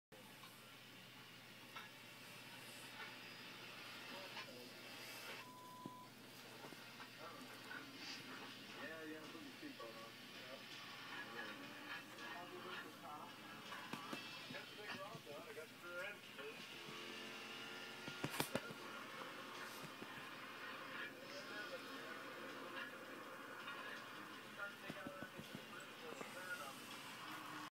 My dogs eye has a scar on it
I just noticed this my dogs fight all the time but i didnt know they bite each others eyes it looks like